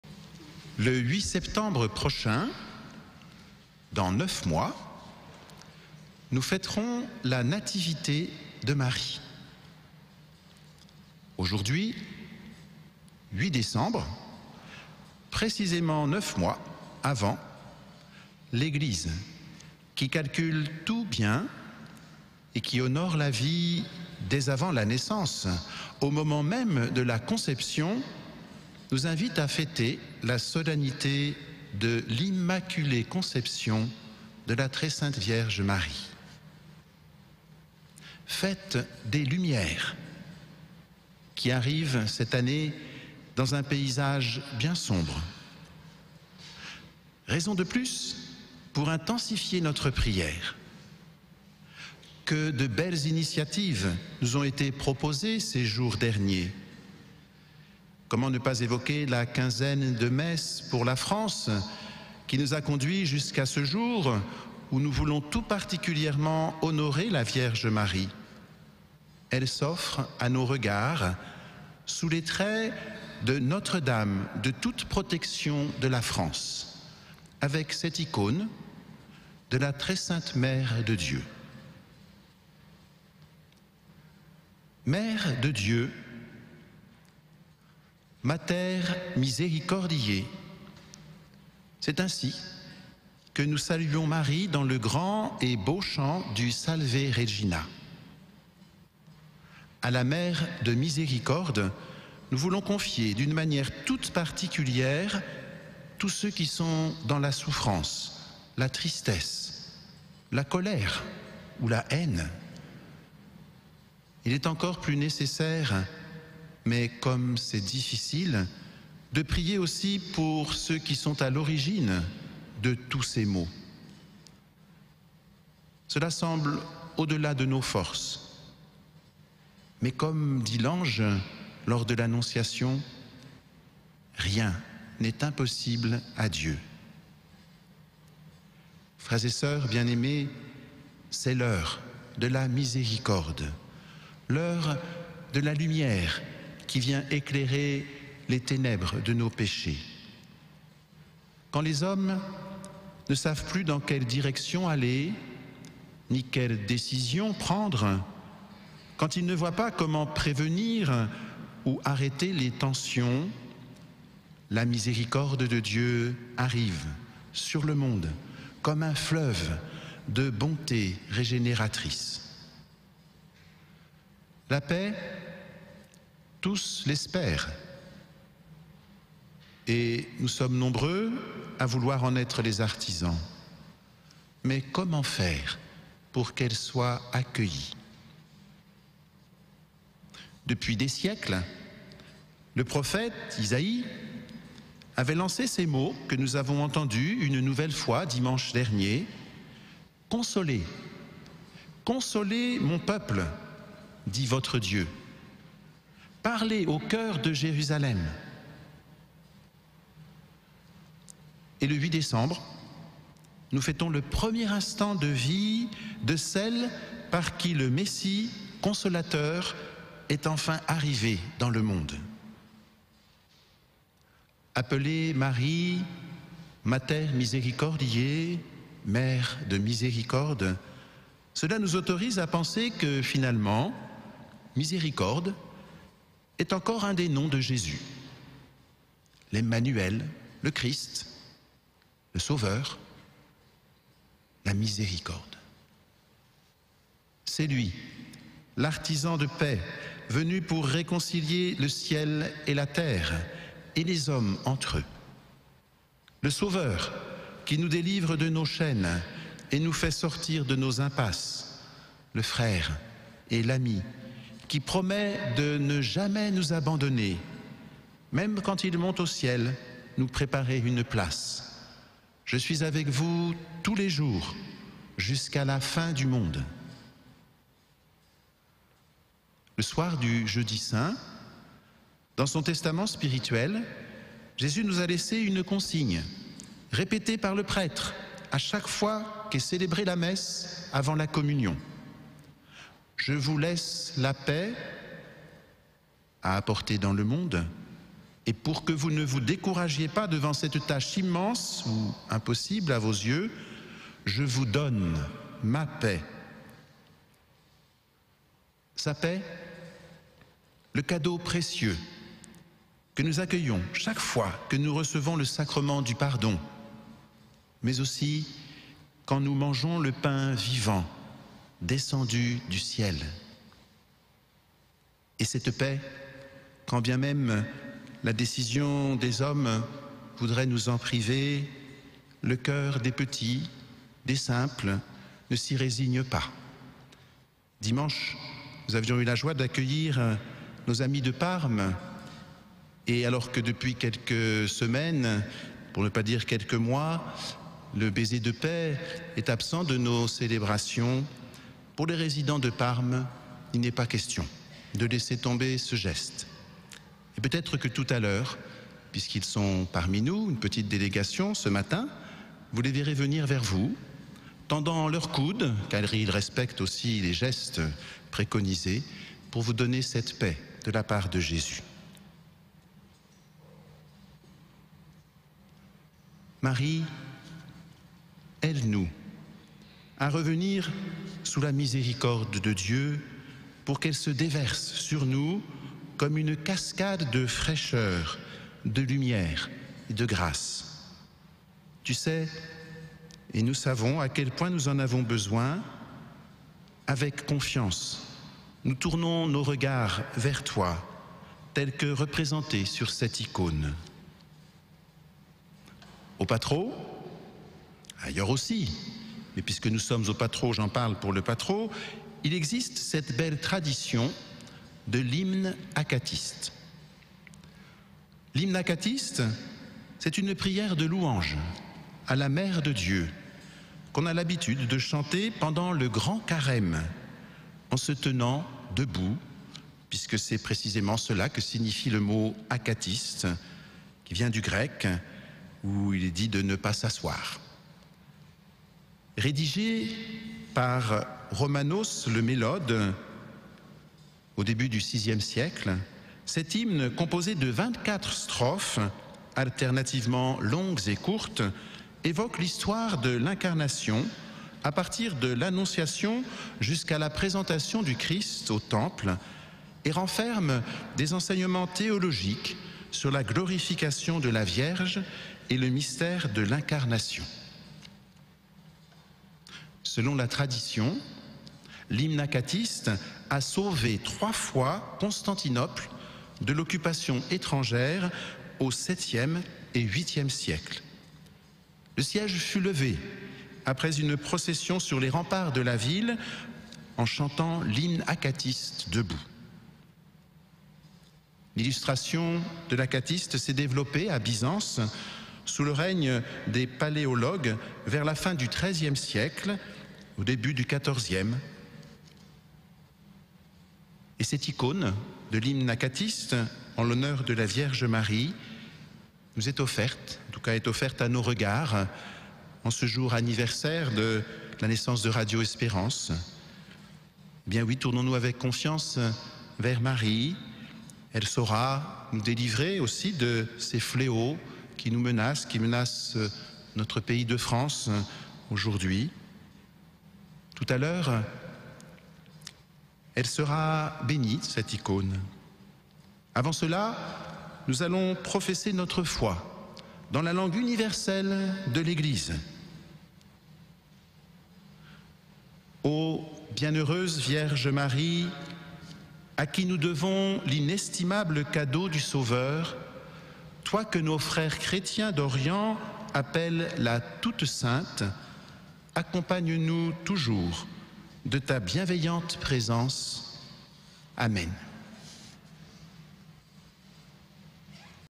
Solennité de l'Immaculée Conception - Homélie du père Dominique Dusang
Homélie prononcée le 08 décembre 2020, à la Chapelle de l'Immaculée Conception à Saint-Etienne